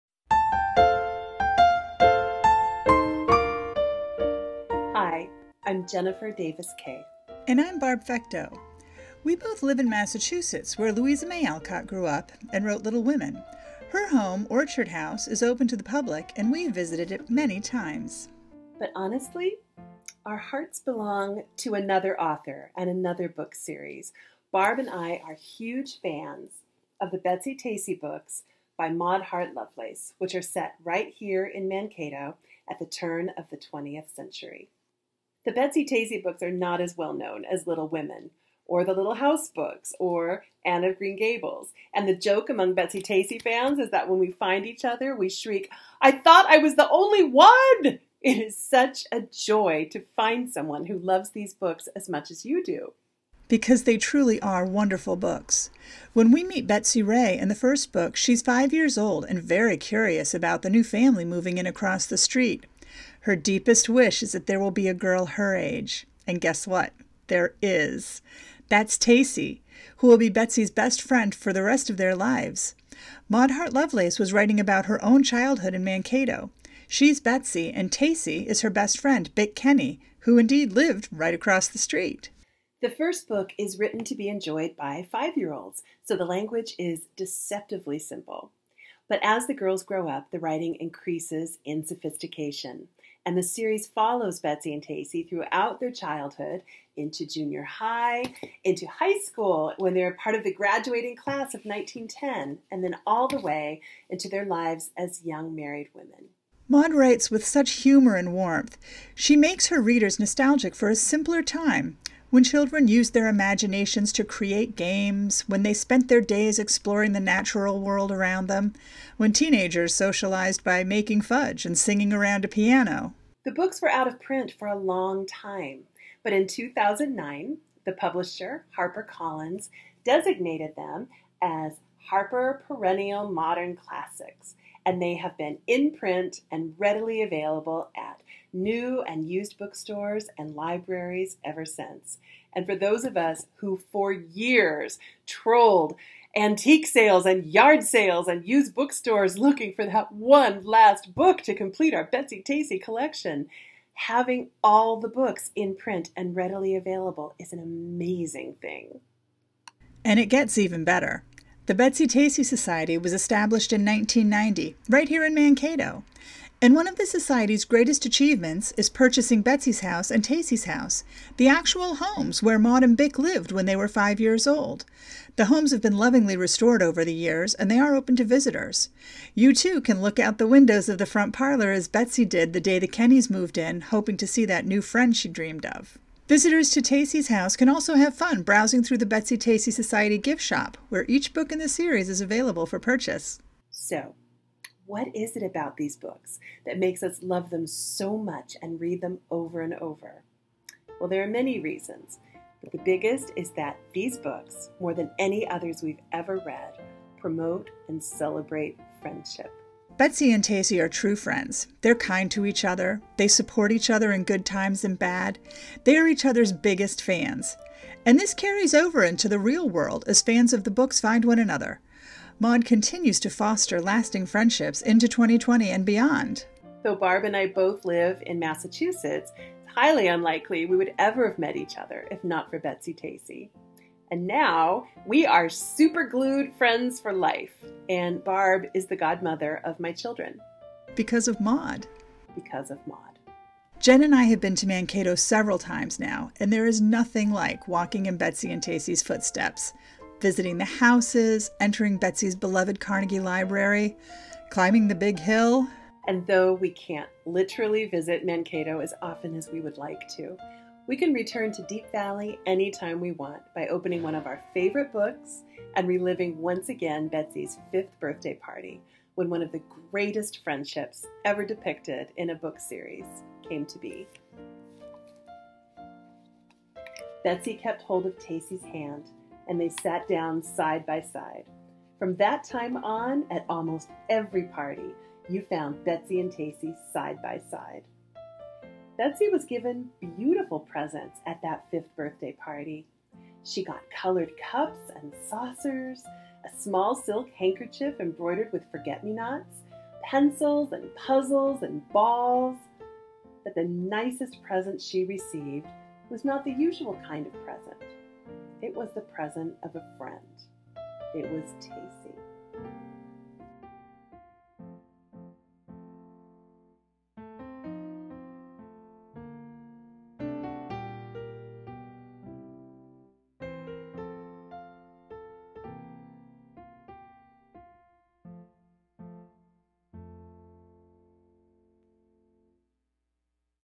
The Betsy-Tacy Books by Maud Hart Lovelace
The Betsy-Tacy series, set in Mankato, Minnesota, at the turn of the 20th century, inspires devotion in readers of all ages around the world. Two longtime fans and Betsy-Tacy Society members – Jennifer Davis-Kay and Barb Fecteau – discuss their love for these books and why they believe Maud’s writing continues to resonate after so many decades.
The Betsy-Tacy illustrations by Lois Lenski and Vera Neville are used with permission from HarperCollins Publishers.